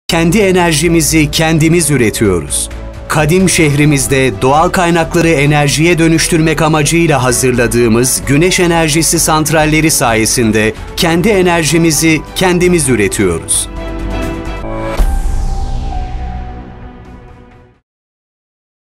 KENDİ ENERJİMİZİ ÜRETECEĞİZ
Kadim şehrimizde doğal kaynakları enerjiye dönüştürmek amacıyla hazırladığımız Güneş Enerjisi Santralleri sayesinde kendi enerjimizi kendimiz üretiyoruz.